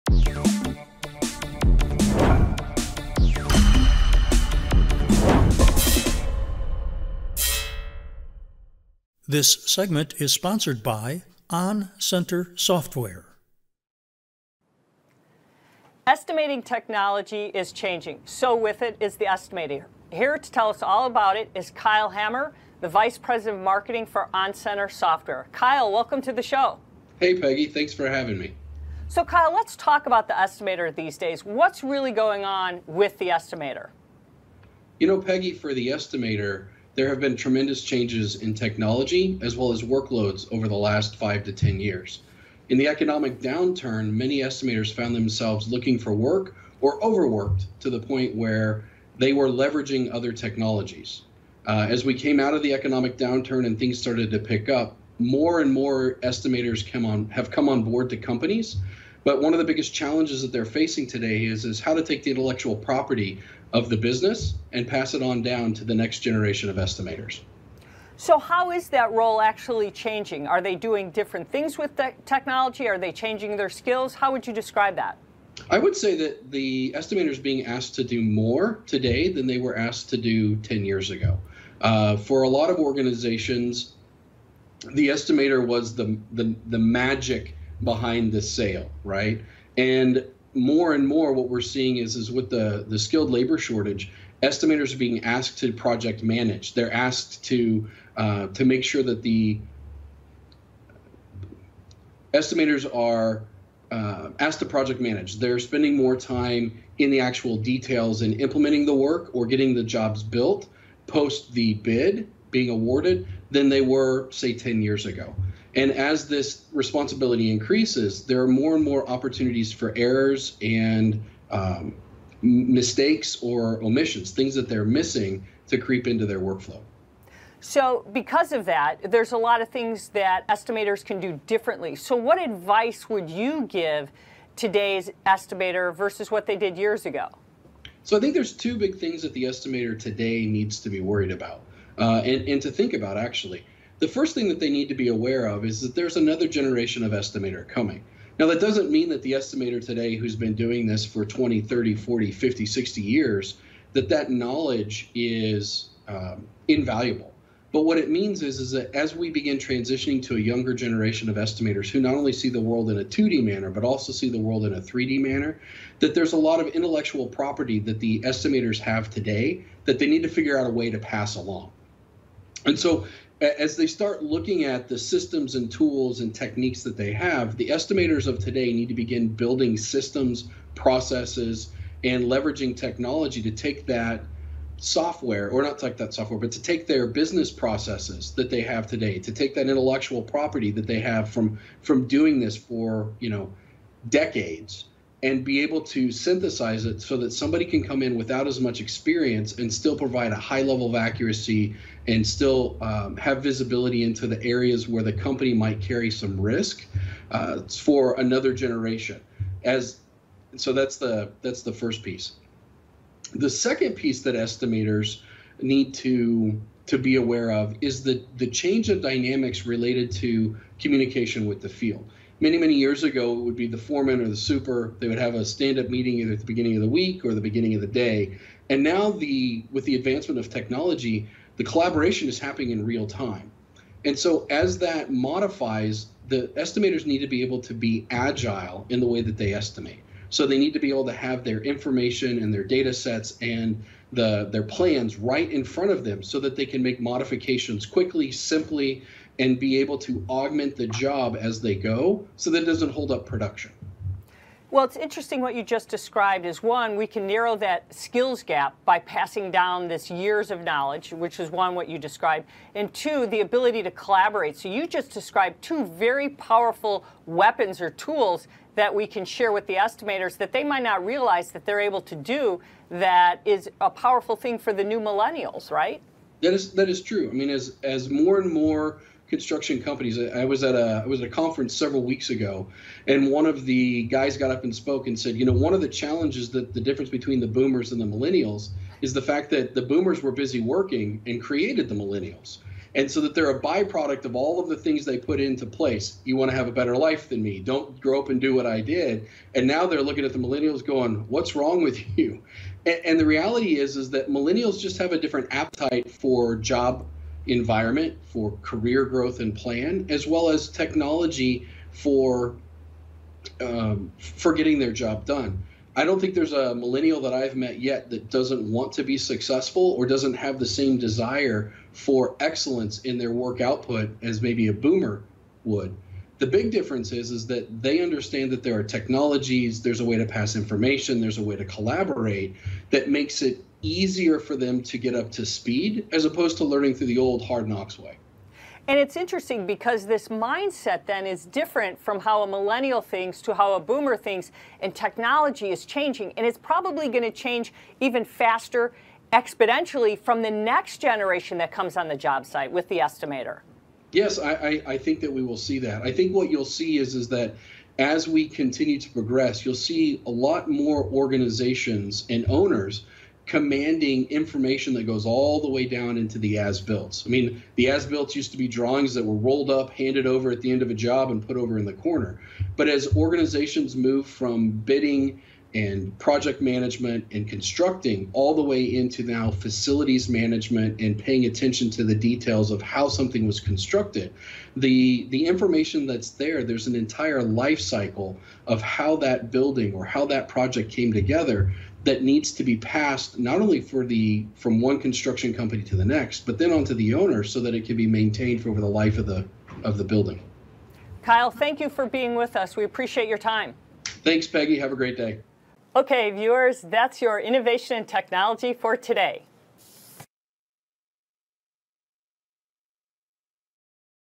Innovation in Technology, On Center Software
Estimating technology continues to evolve—and with that the role of the estimator needs to change as well. Kyle Hamer, vice president of marketing, On Center Software, joins Peggy Smedley to discuss the evolving job of the estimator.